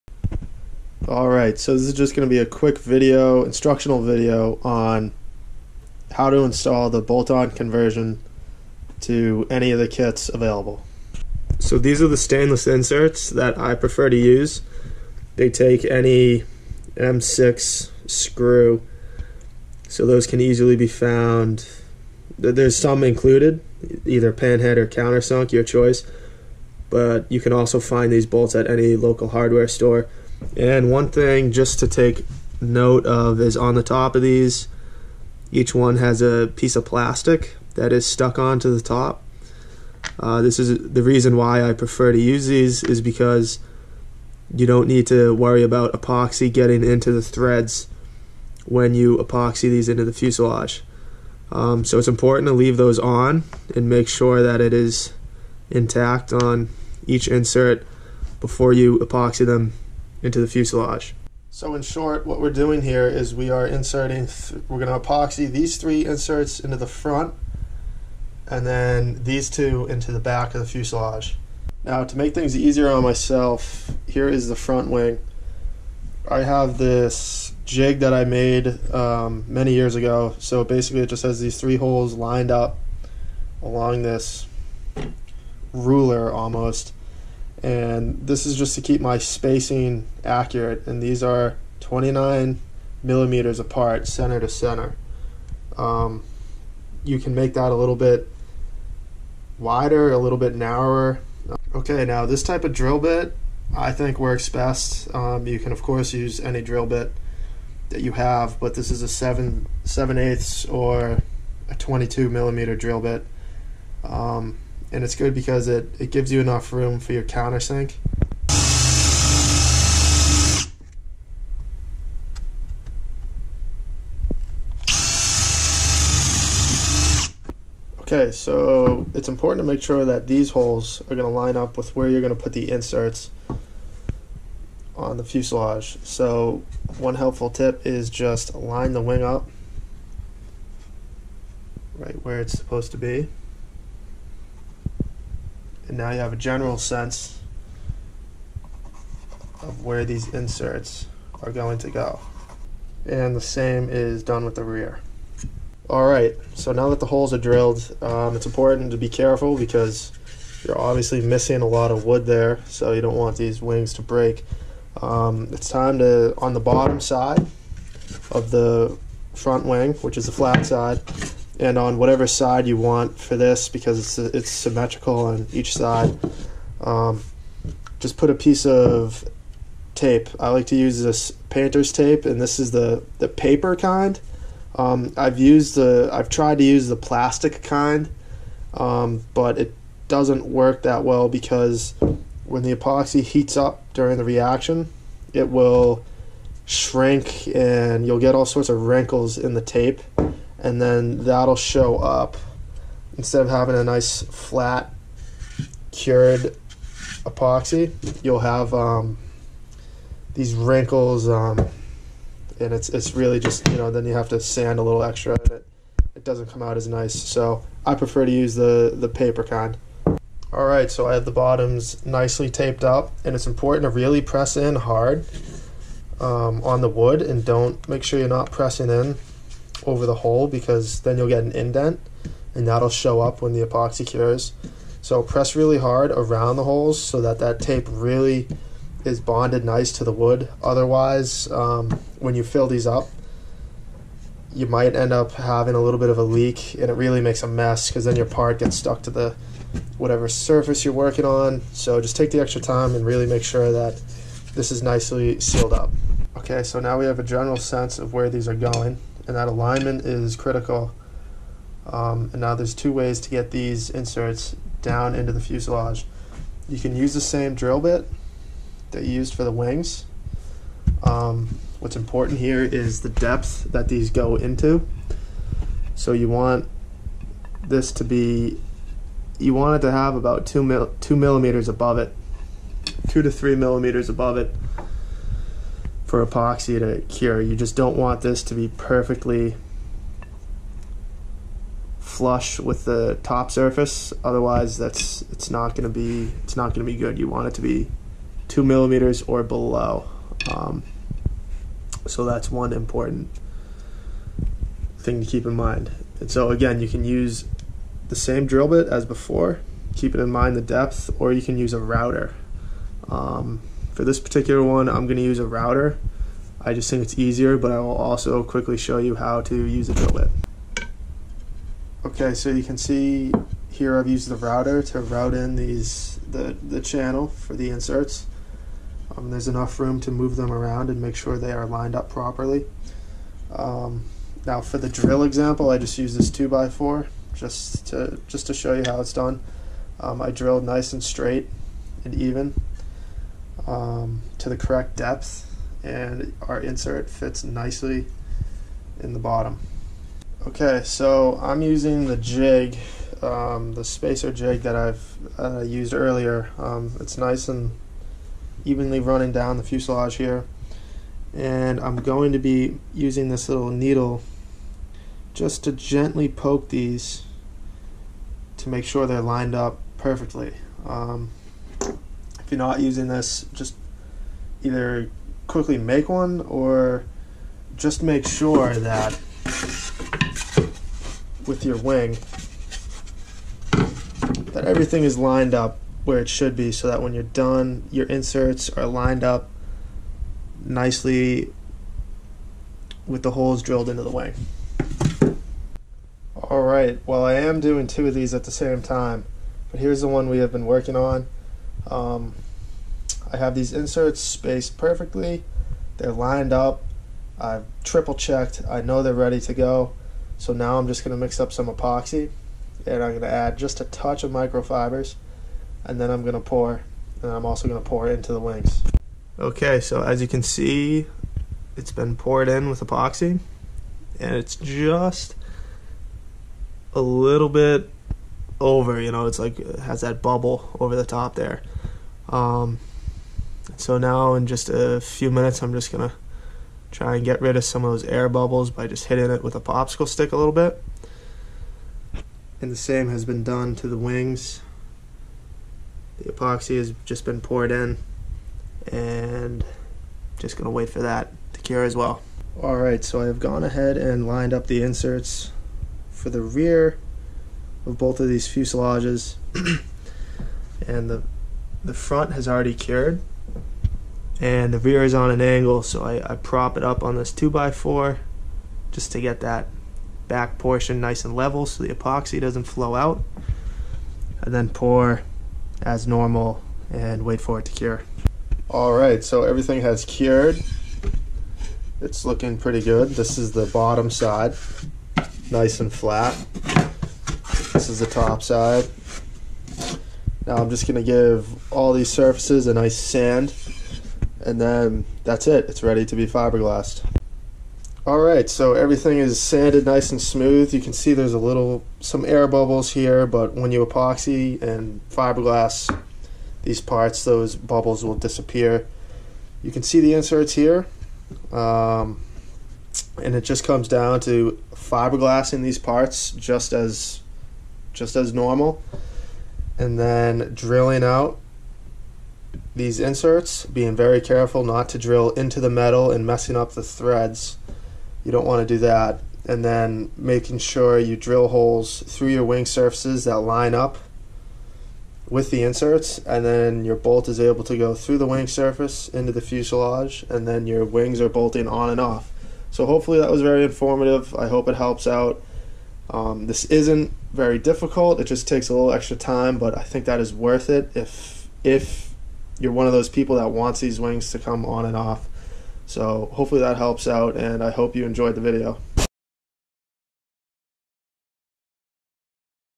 HOW TO Make Removable Hydrofoil Wings
This is a subject that many people have asked about. Hopefully this video explains everything you need to know about installing the bolt on hardware into your standard, sup, or superwing kit.